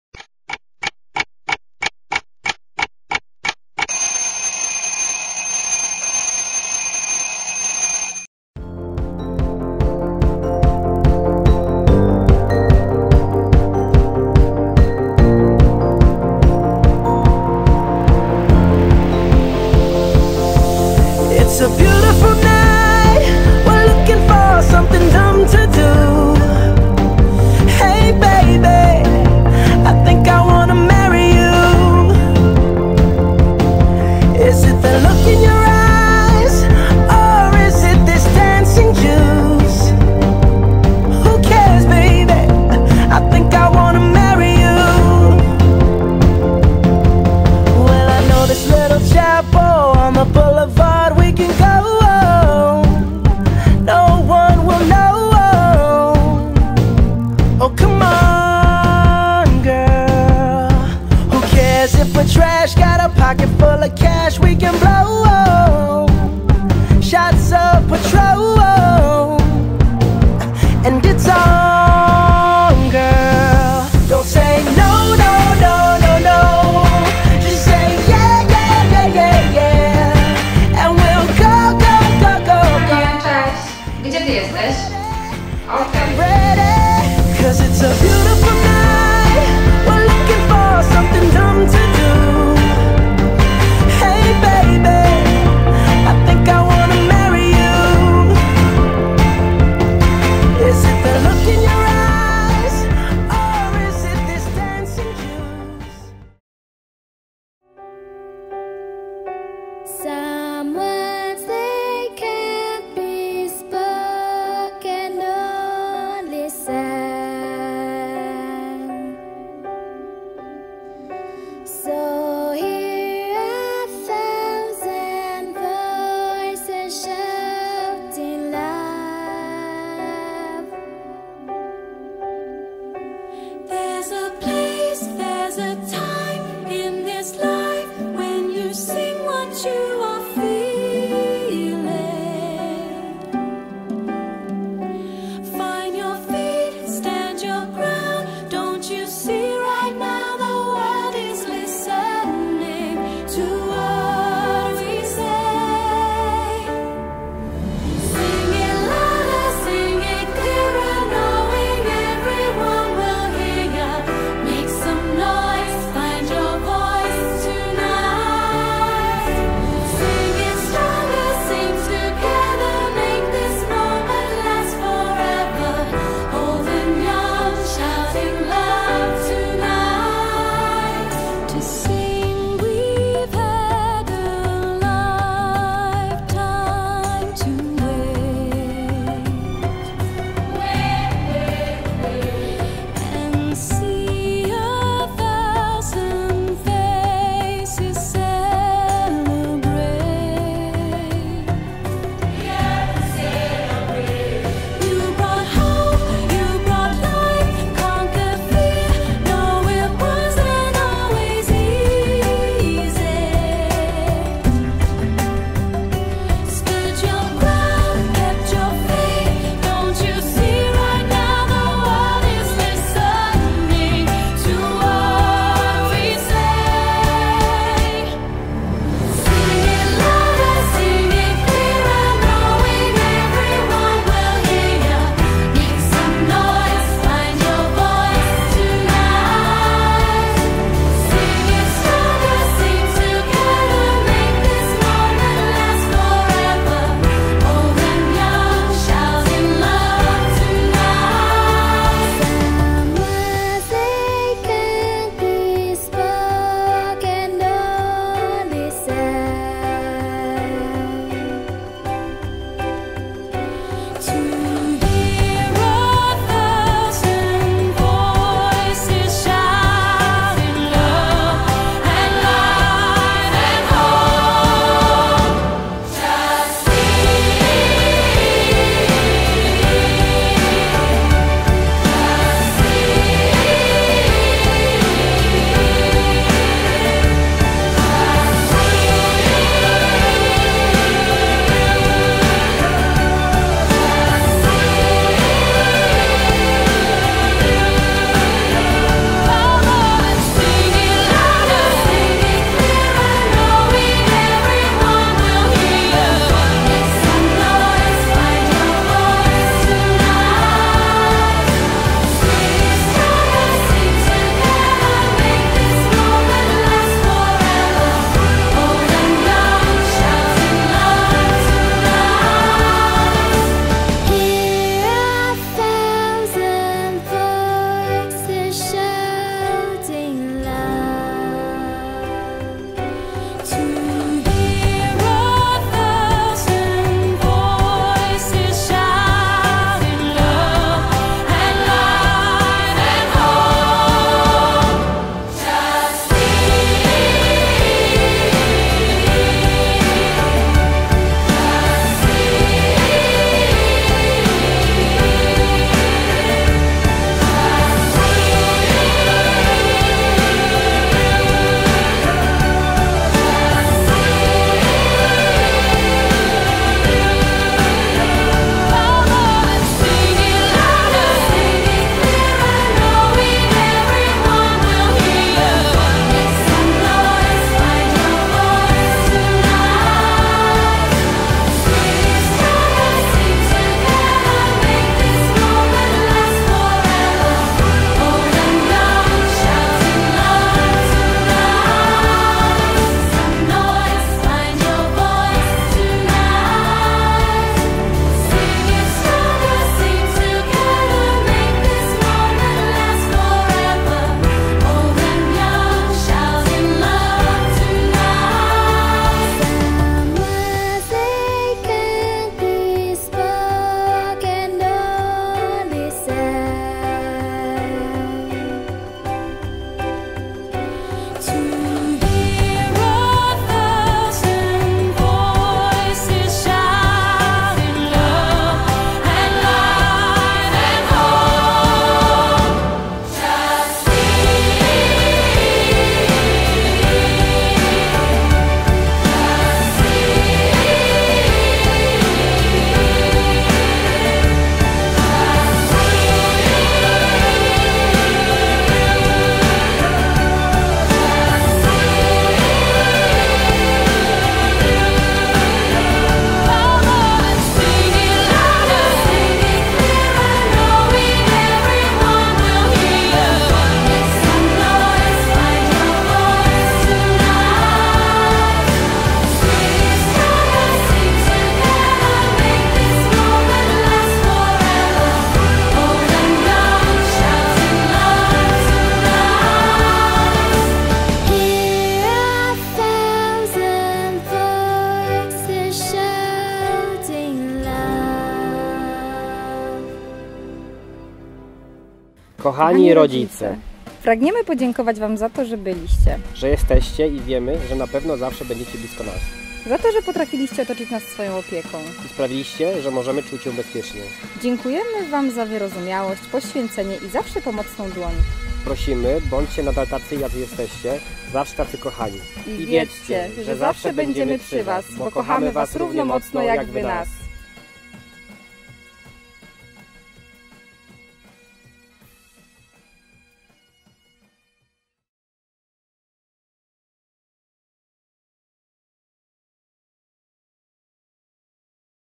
Podziękowania dla rodziców Sylwii i Waldka
Teledysk z podziękowaniami dla rodziców Sylwii i Waldka. Ślub odbył się 23 sierpnia 2015